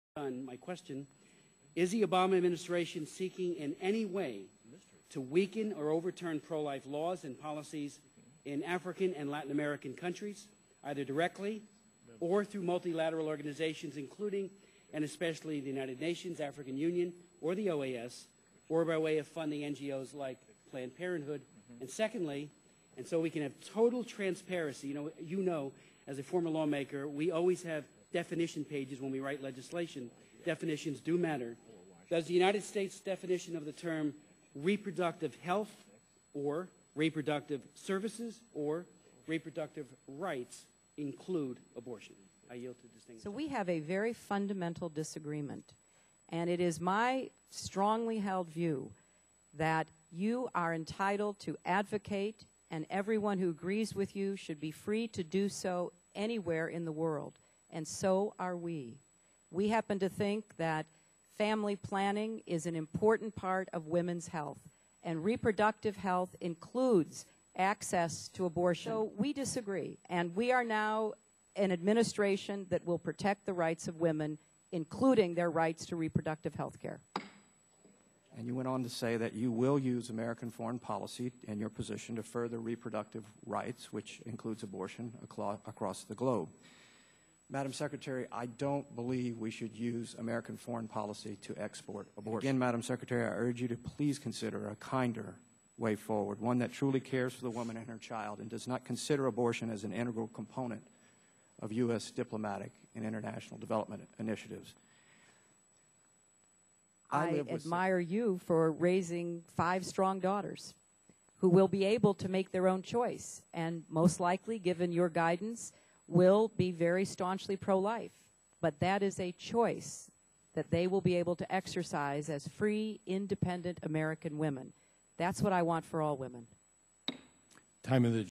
2009 U S Policy to Export Abortion (Rep. Chris Smith and Sec. Clinton)
Rep. Chris Smith and Hilary Clinton discuss US Policy with respect to abortion.